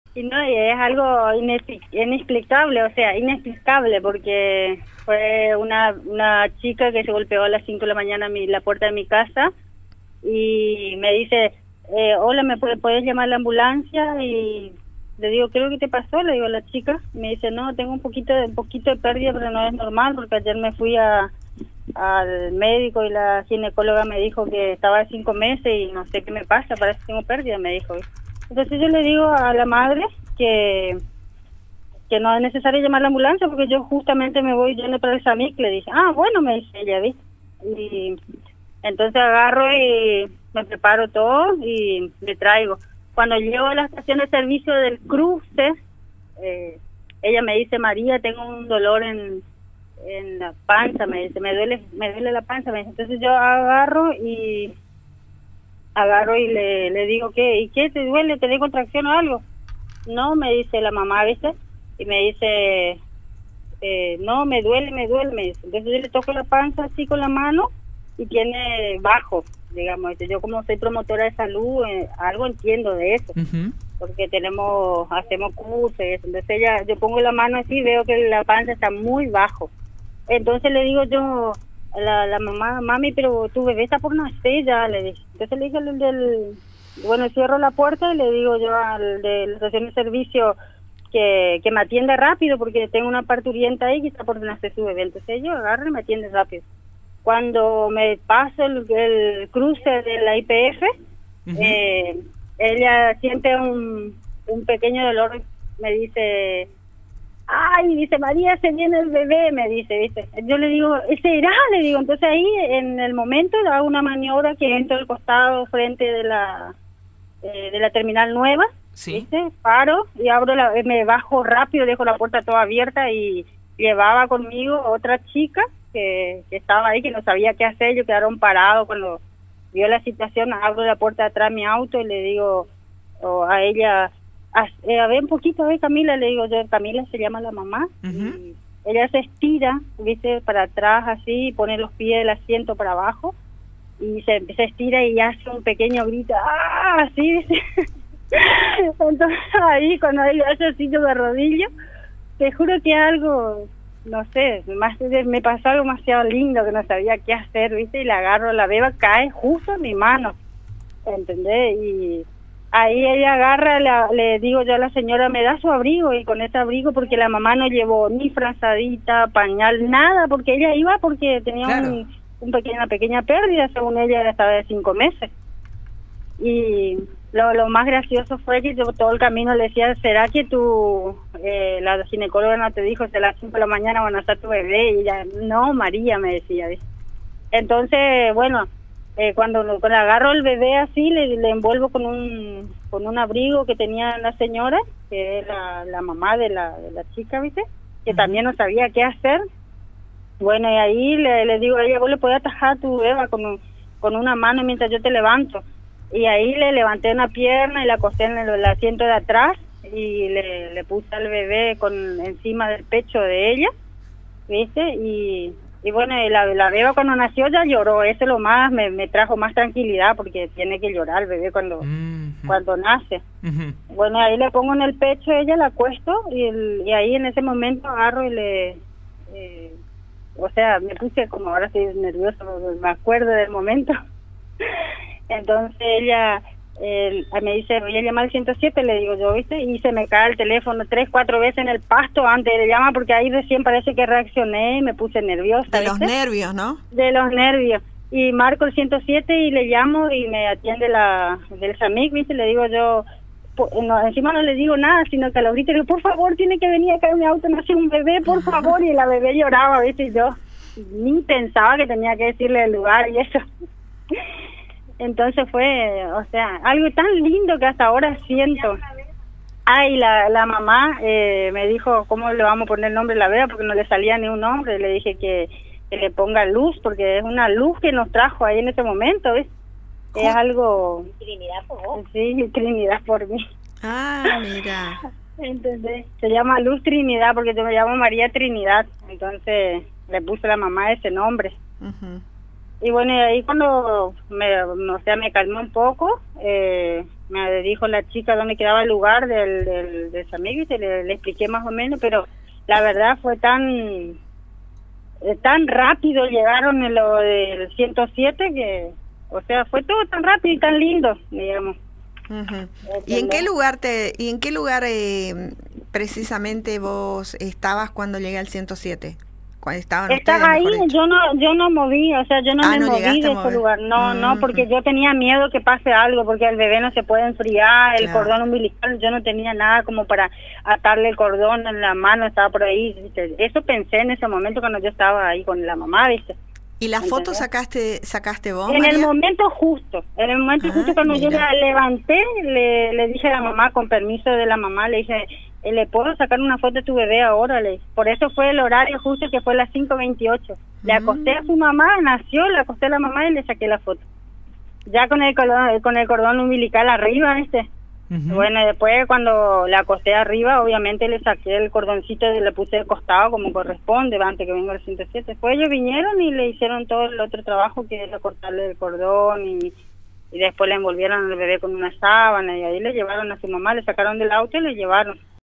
María vivió un momento inolvidable: una beba nació en sus brazos
María Trinidad Romero fue protagonista de un hecho que jamás hubiera esperado le pasara en su vida: ser partícipe del nacimiento de una beba.